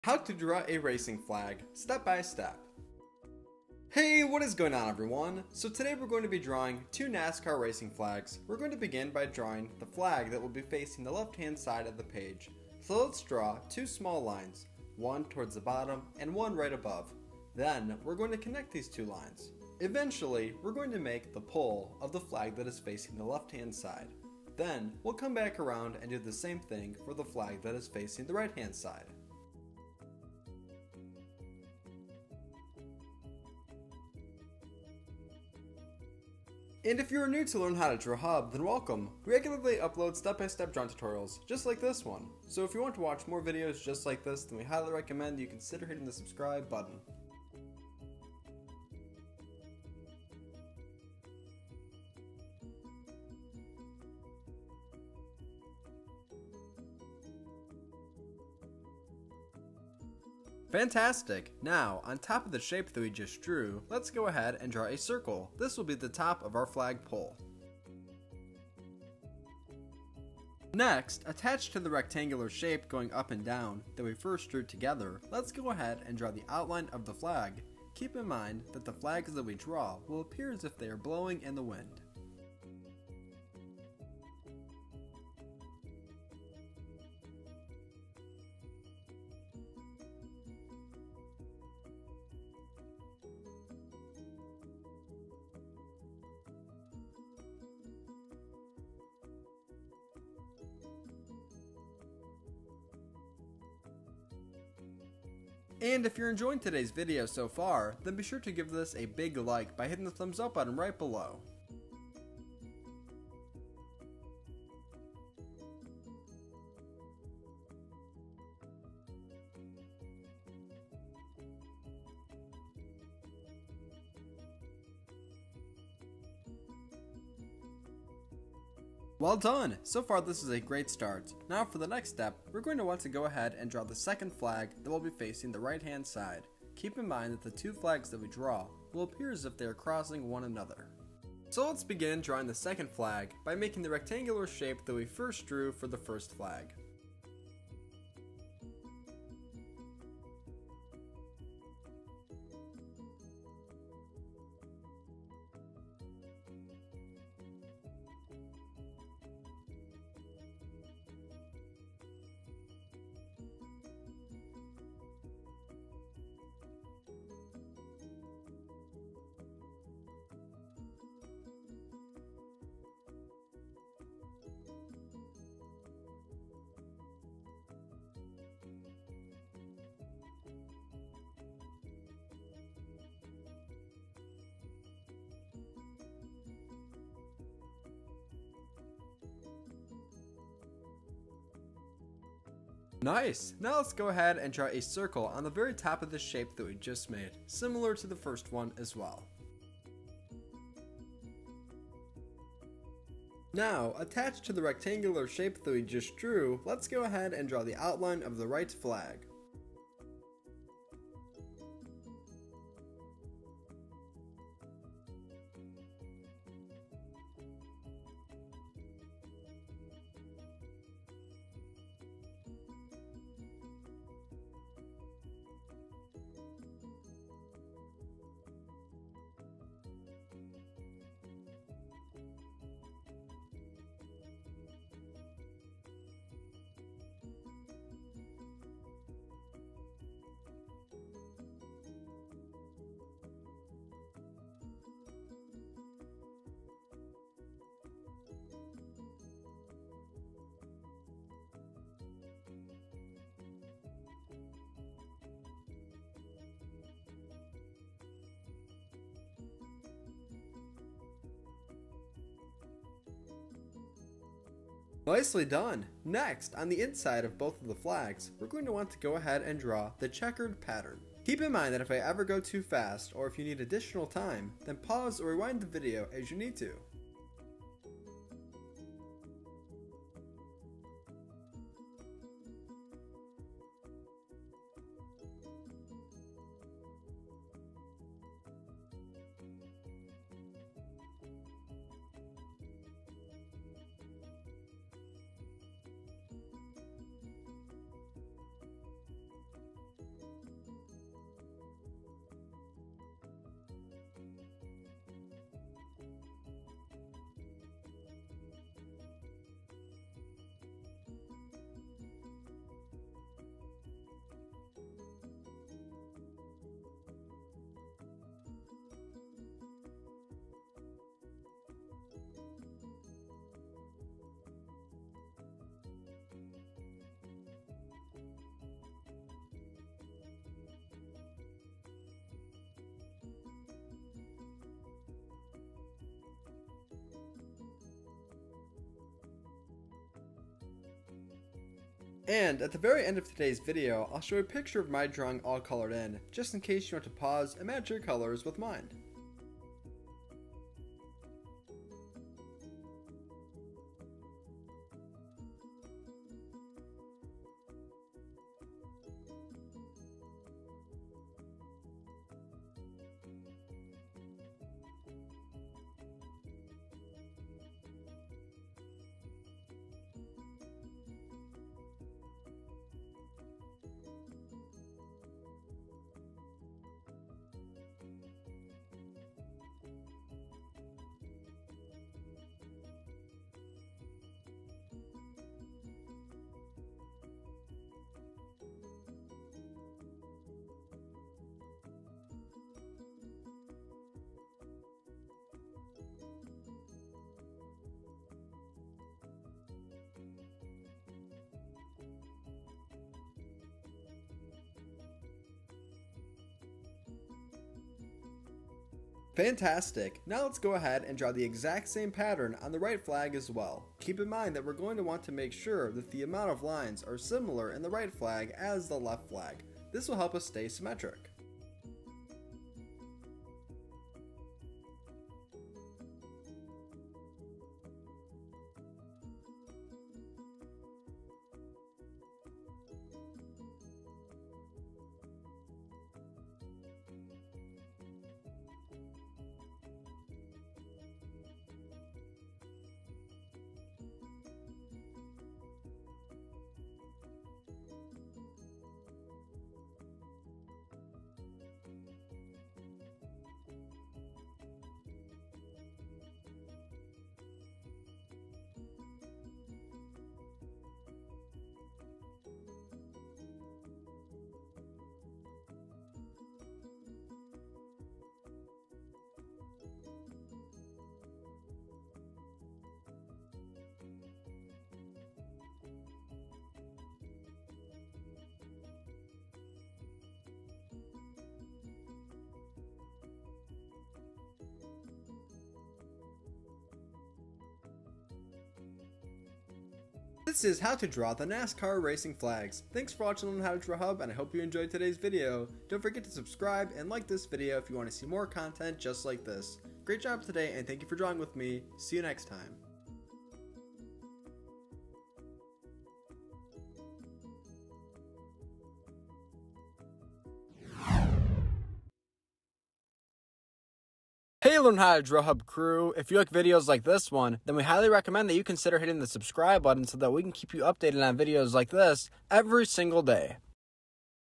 How to Draw a Racing Flag (Nascar Racing Flag)
Calling all NASCAR fans and aspiring artists!  Ever dreamed of drawing those iconic black and white checkered flags waving at the finish line? Look no further! This step-by-step tutorial will guide you through the process, from sketching the basic shapes to adding dynamic folds and shadows.